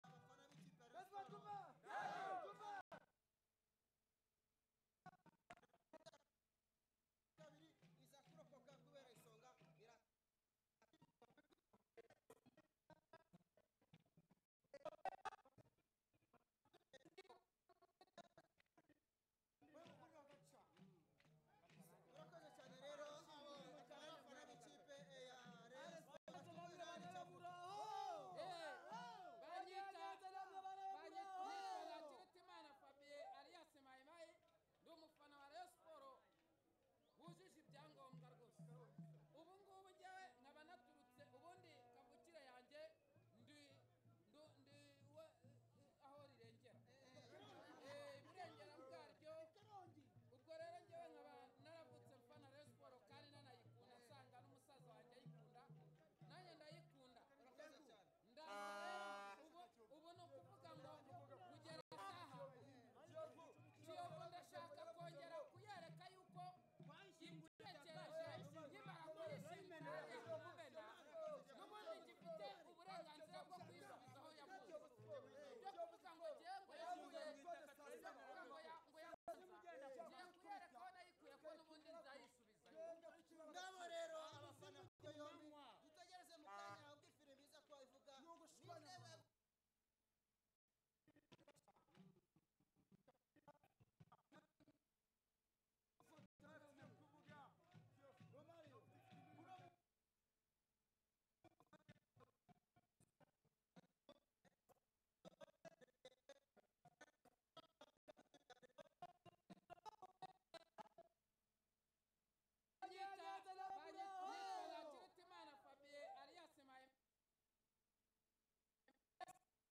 INNOCENT
Habyarimana Innocent in warm-up Day 2 Azam Rwanda Premier League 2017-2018